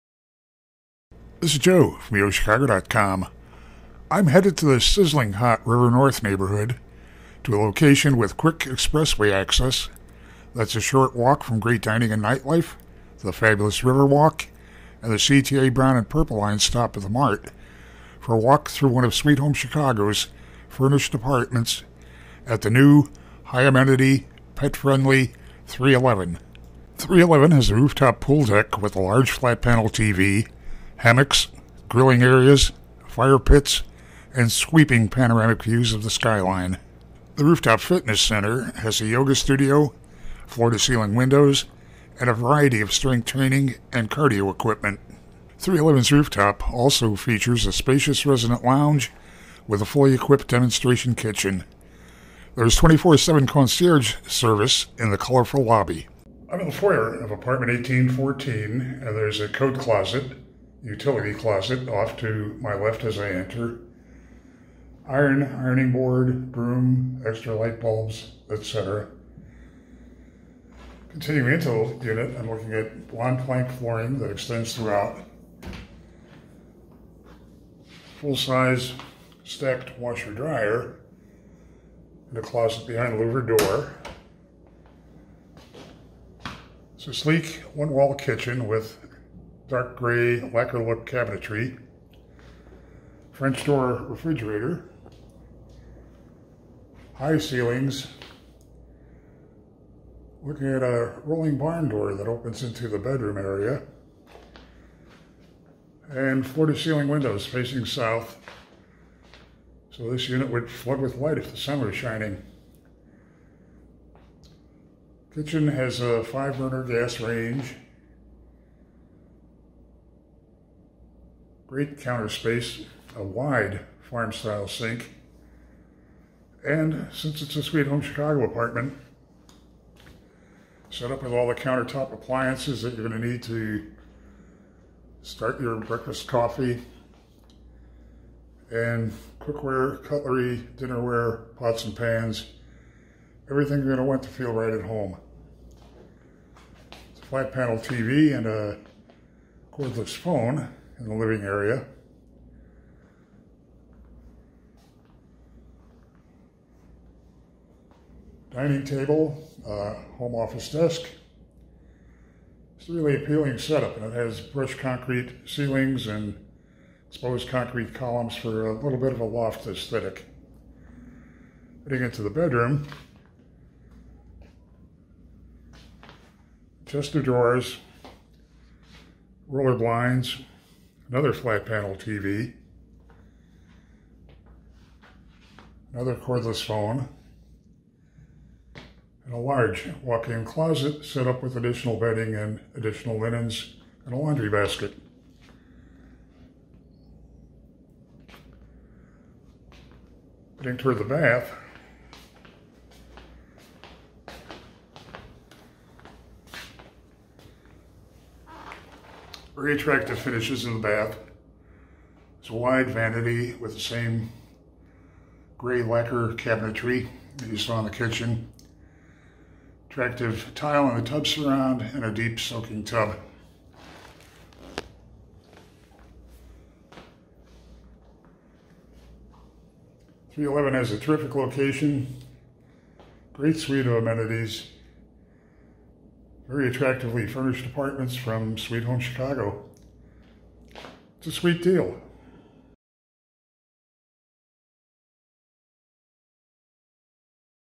River North furnished 1-bedroom #1814 near the Riverwalk and theMART
3Eleven is the newest full-amenity apartment tower in the hot River North neighborhood. It's close to the Merchandise Mart, the Riverwalk, the Loop and public transportation.

3Eleven apartments have upscale finishes, floor-to-ceiling windows, and wide plank flooring throughout, and in-unit washer / dryers.

Suite Home Chicago apartments are set up with everything you need to feel at home, and hotel-like services to ensure your comfort. It's a suite deal.

Join YoChicago in this sponsored video for a narrated walk through one of Suite Home Chicago's apartments.